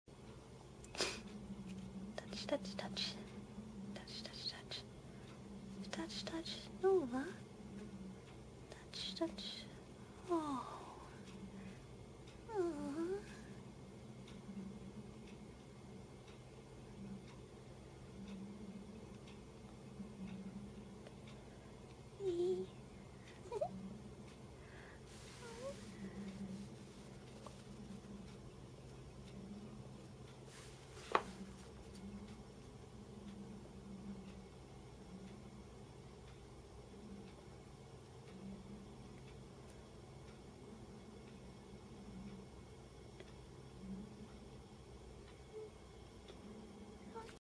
Nova the white chinchilla loves to be loved!
This is my chinchilla, Nova! I thought he looked so silly standing at the corner of his cage, so I went over and started to rub his little chin! He loved it!